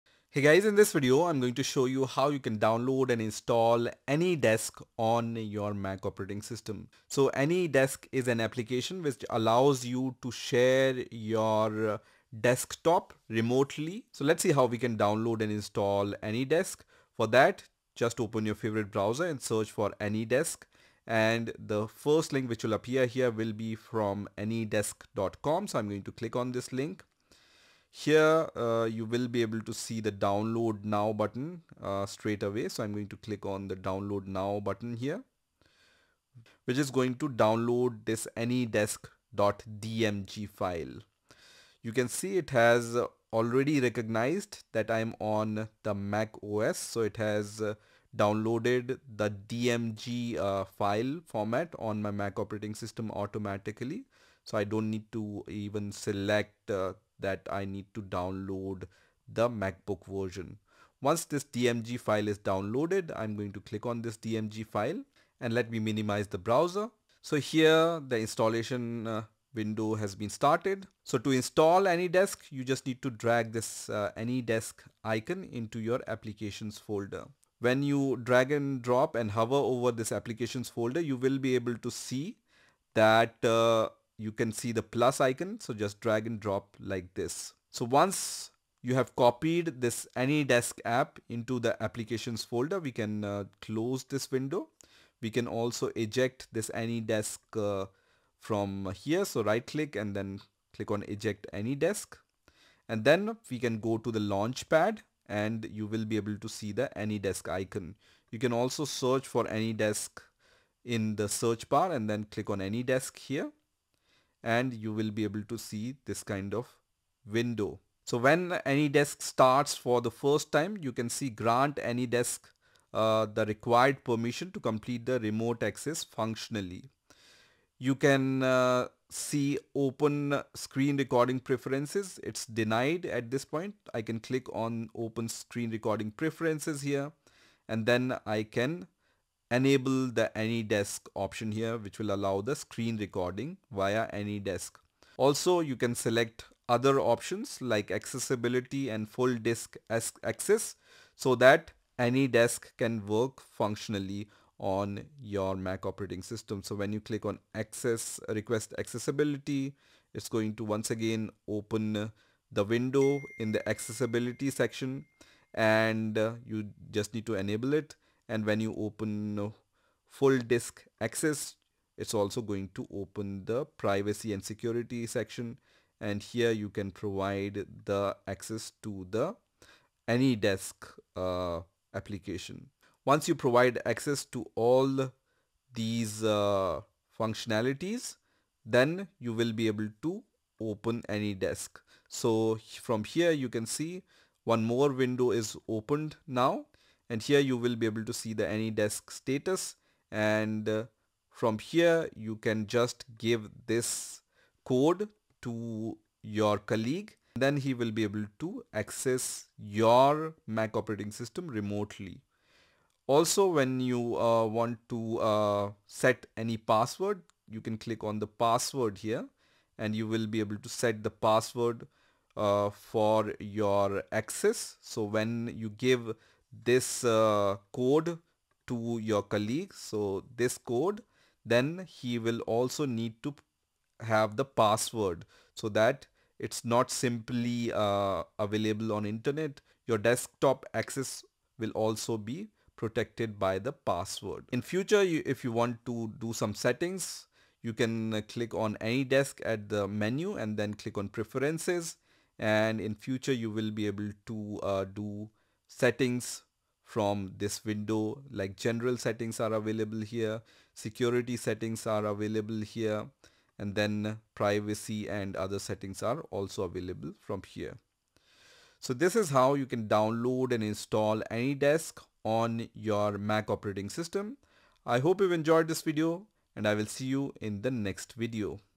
How to Install AnyDesk on Mac | Remote Access Setup on macOS Sonoma/Ventura
🖥️ Looking to remotely access and control other computers using your Mac? In this easy 2025 step-by-step tutorial, I’ll show you how to download, install, and use AnyDesk on macOS—perfect for remote work, tech support, or accessing your own devices.

AnyDesk is a fast and secure remote desktop tool that works seamlessly on MacBook, iMac, and Mac Mini, whether you're using macOS Sonoma, Ventura, or Monterey on Apple Silicon (M1, M2, M3) or Intel chips.

💡 In this tutorial, you’ll learn:

✅ How to download the official AnyDesk installer for Mac
✅ How to install and give screen recording & accessibility permissions
✅ How to launch AnyDesk and share your address
✅ How to accept or request remote access securely
✅ BONUS: Tips on improving speed and setting passwords for unattended access

📌 Setup Steps:

Download the .dmg file from the AnyDesk website

Open it and drag the AnyDesk app to Applications

Launch the app and allow screen and accessibility permissions

Share your AnyDesk address to allow connections

(Optional) Set password for unattended access

🧠 Ideal For:

Remote workers and freelancers

IT support professionals

Accessing a home or work computer on the go

Secure remote desktop sharing with family or team